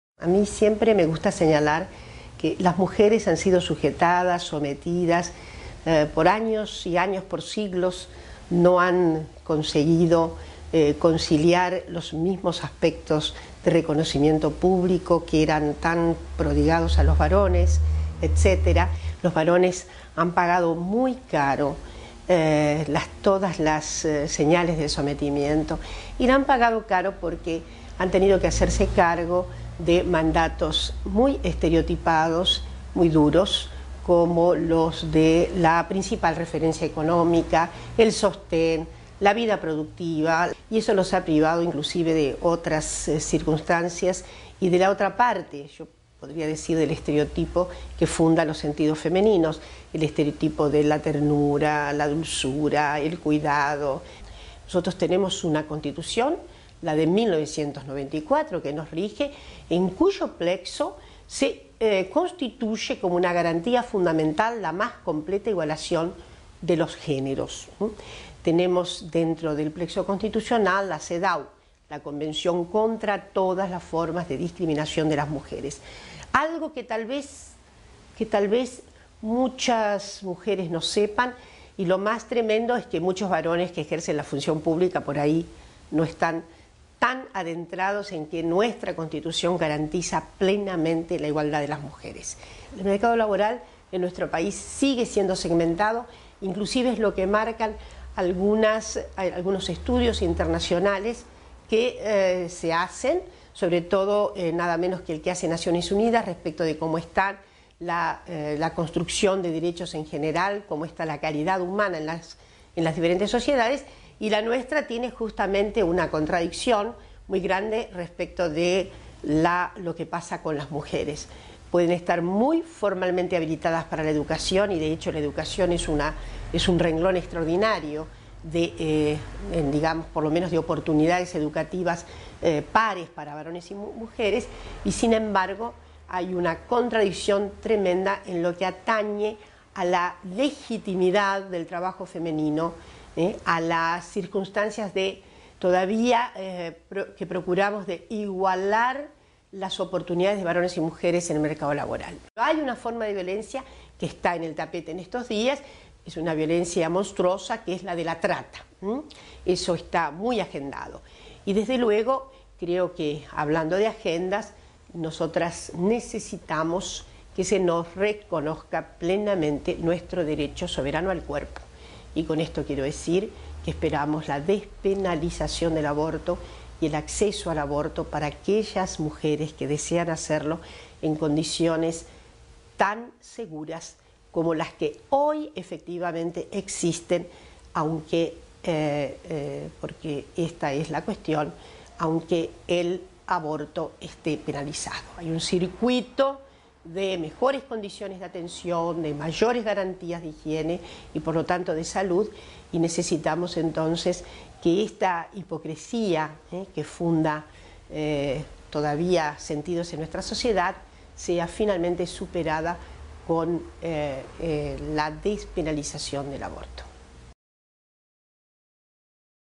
Entrevista a Dora Barrancos
"Las mujeres no hemos podido conciliar los mismos aspectos de reconocimiento público que los varones"

En el marco del Día Internacional de la Mujer, la doctora en ciencias humanas e investigadora del Conicet Dora Barrancos destacó que "las mujeres no hemos podido conciliar los mismos aspectos de reconocimiento público que los varones". Con respecto a los mandatos culturales sobre los roles, dijo que "los varones han tenido que hacerse cargo de mandatos muy estereotipados".

Barrancos agregó que "nuestra Constitución garantiza la igualación de los géneros, sin embargo no hay aún igualdad de oportunidades entre mujeres y varones en el mercado laboral" y remarcó que "las mujeres necesitamos que se reconozca plenamente nuestro derecho soberano al cuerpo"